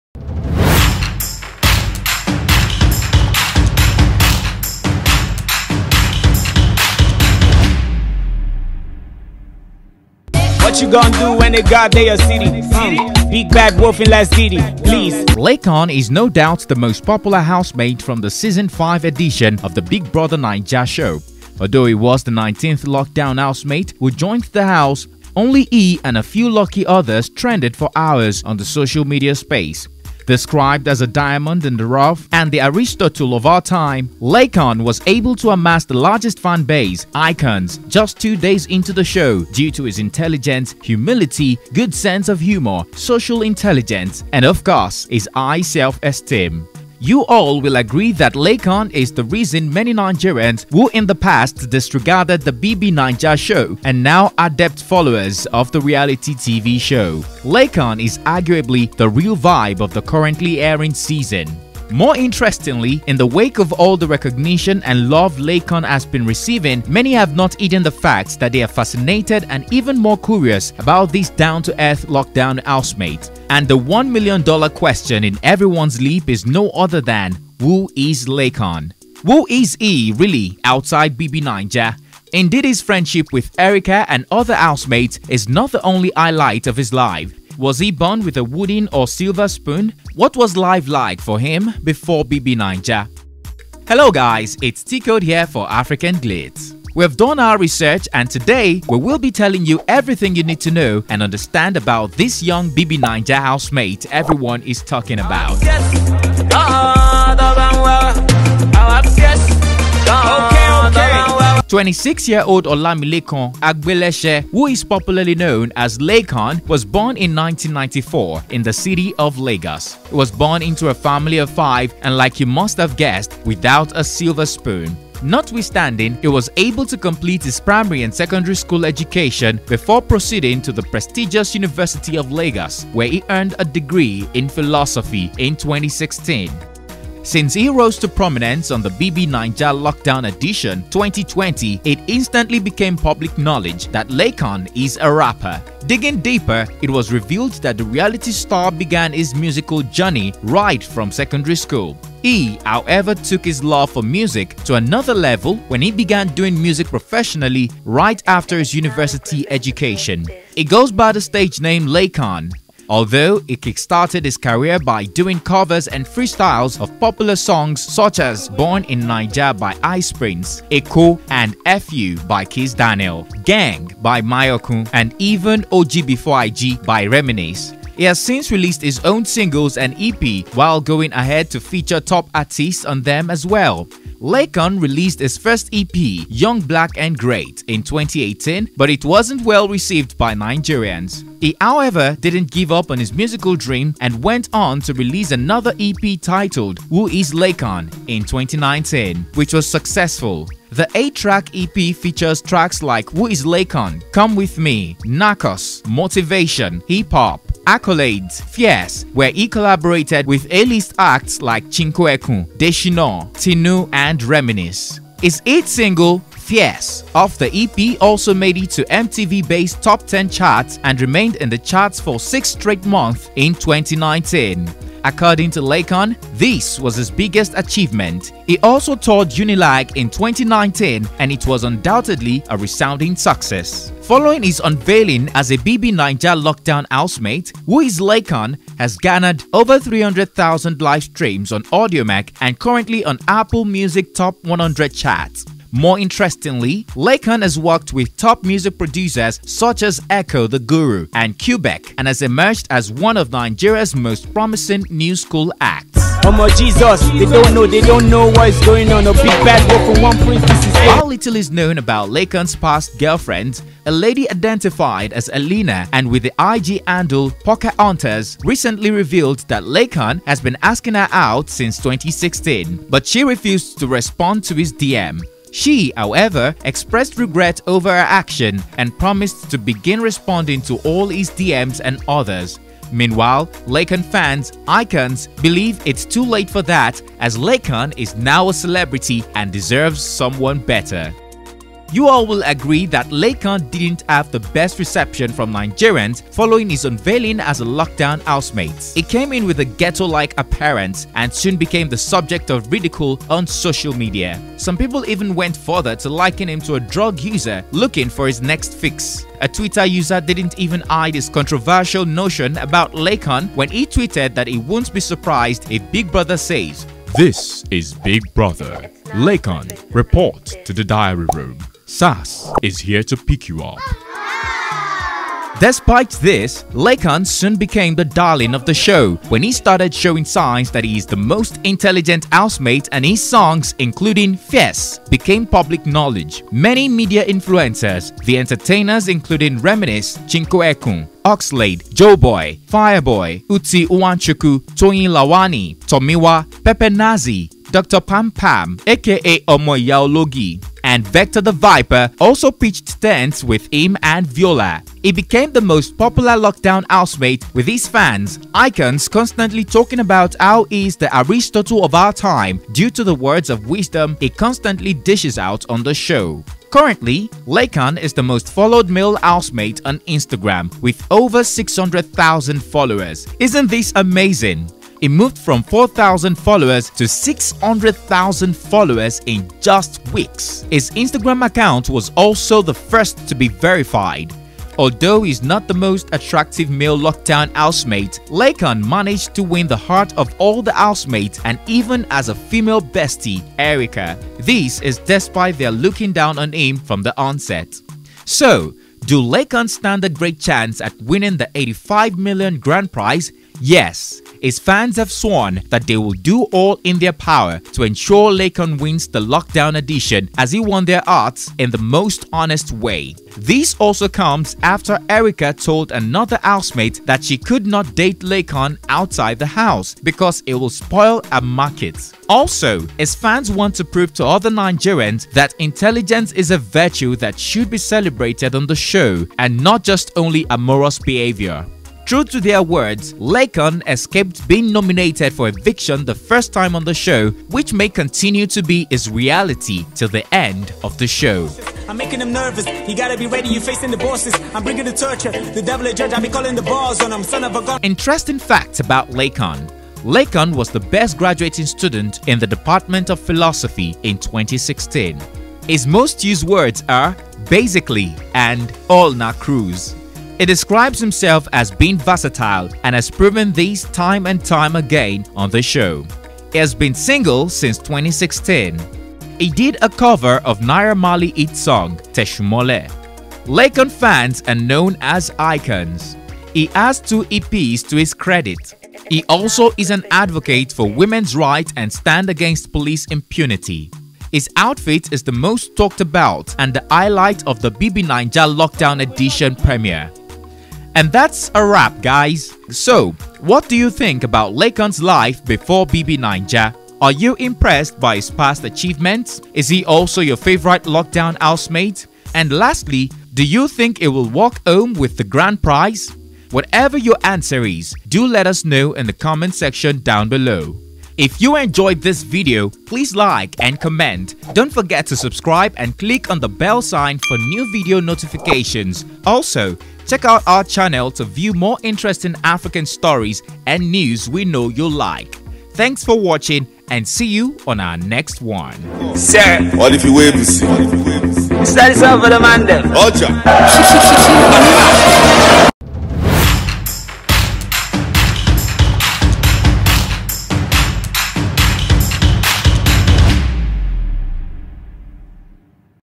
THE STORY OF LAYCON - CAN HE WIN BIG BROTHER NIGERIA 2020?
Olamilekan Agbeleshe who is popularly known as Laycon was born in 1994 in the City of Lagos. He was born into a family of five and like you must have guessed without a silver spoon. Notwithstanding, he was able to complete his primary and secondary school education before proceeding to the prestigious, the University of Lagos where he earned a degree in Philosophy in 2016.
Script: Temitope Taiwo
Voiceover: TCode
Video editor: Noellin
Background music Instrumentals.ng